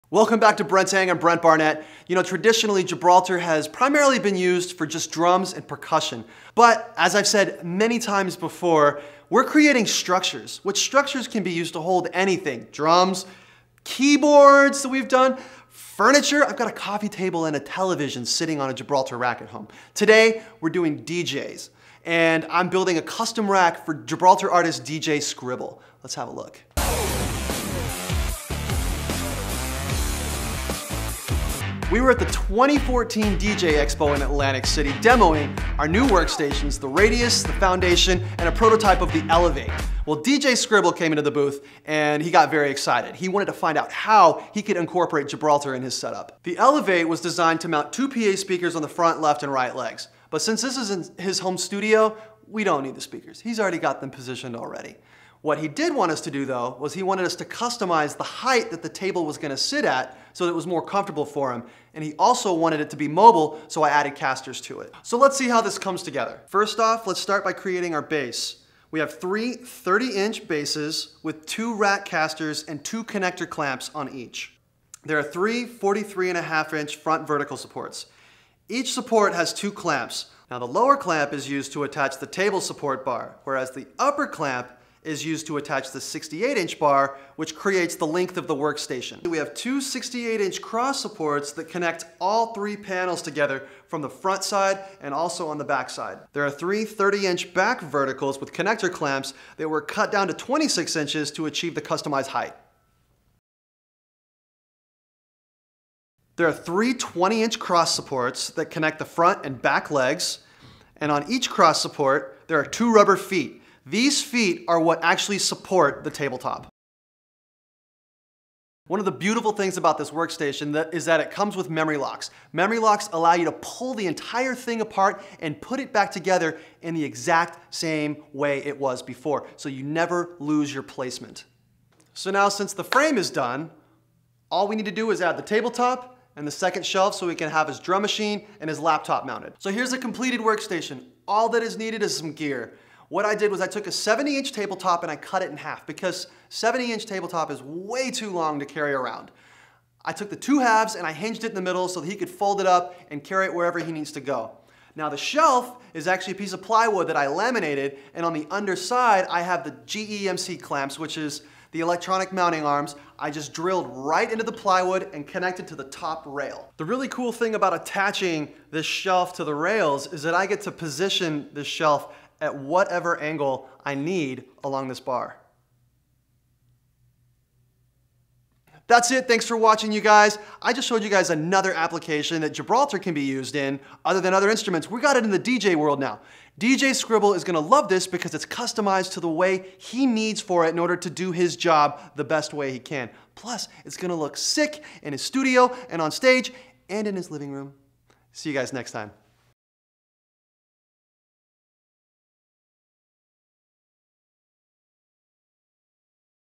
Custom Elevate Workstation for DJ Skribble - Brent's Hang
Brent customized this Elevate DJ Workstation for DJ Skribble​ by changing the height, adding casters and memory locks.